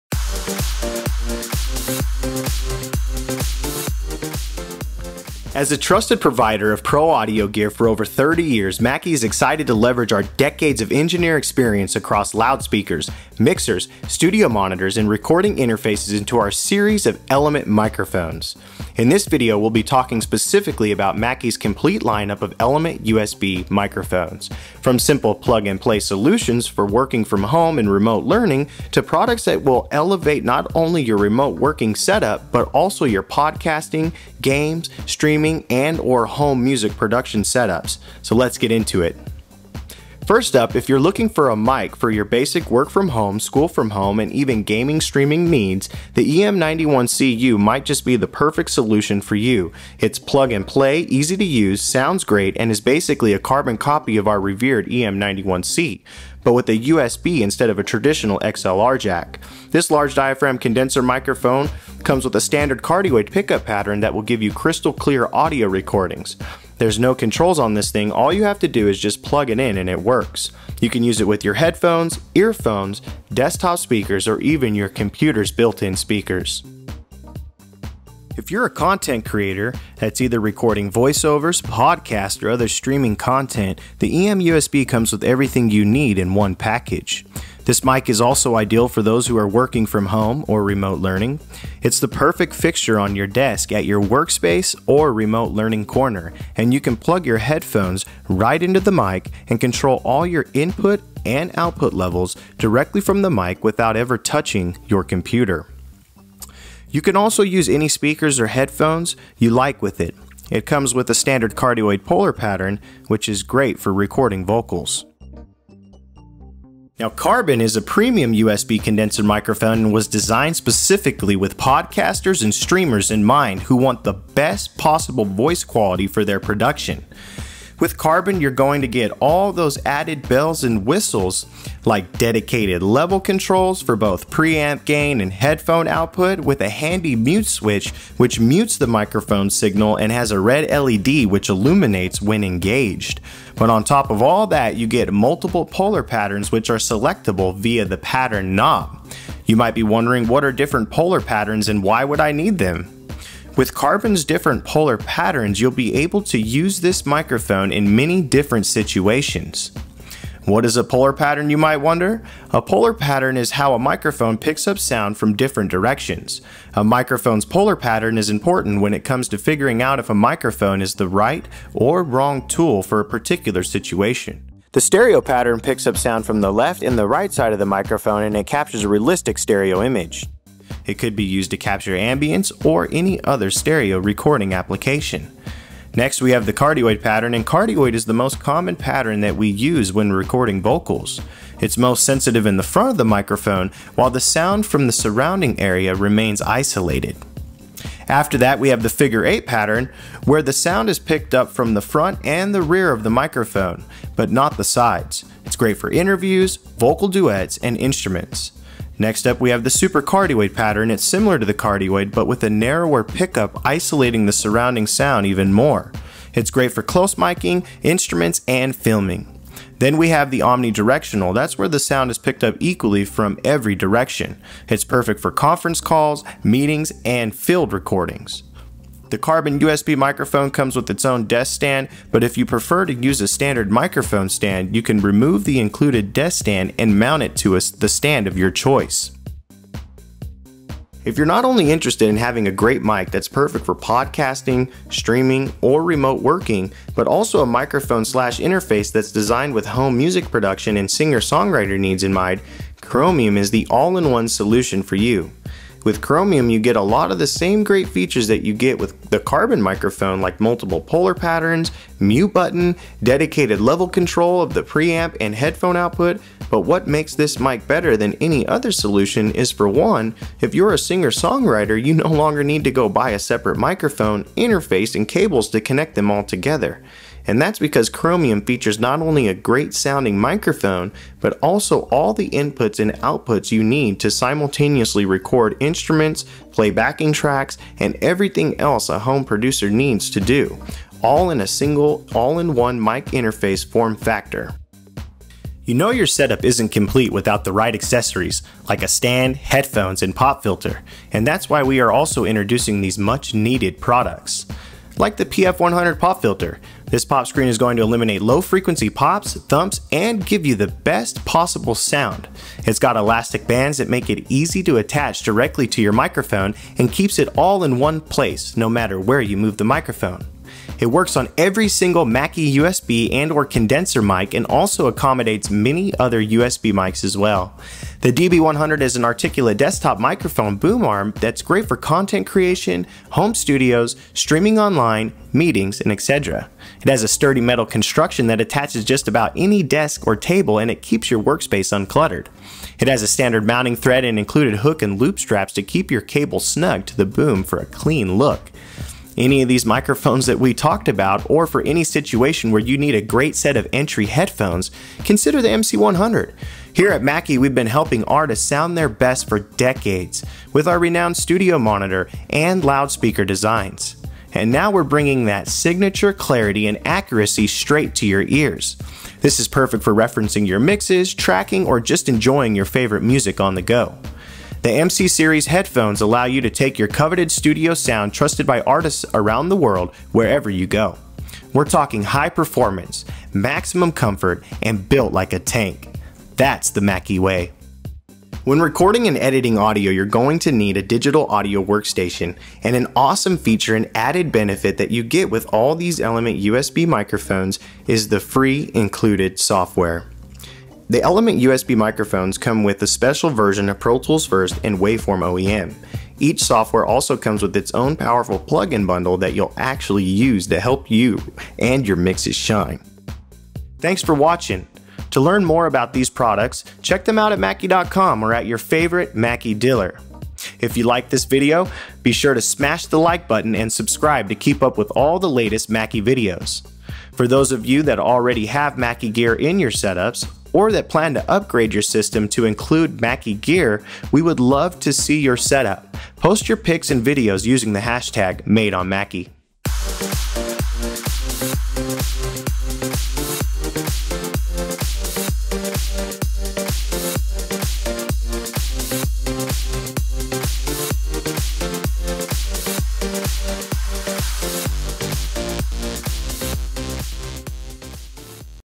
Meet the New EM Series USB Microphones
Whether you're working from home or pushing for the biggest sub train yet on your live stream, how you sound matters. From simple plug-and-play to feature-rich versatility, EM Series has the perfect USB microphone to suit your needs. Join Brad Gagne as he explores the various models and what works best for applications like remote learning, working from home, podcasting, home music production, streaming, content creation, and beyond.

If you want to get to the good stuff...
0:00 Intro
0:39 EM-91CU
1:17 EM-USB
1:56 Carbon
2:53 Learn about Polar Patterns!
4:00 Chromium
4:55 Accessories
6:27 Software
6:53 Outro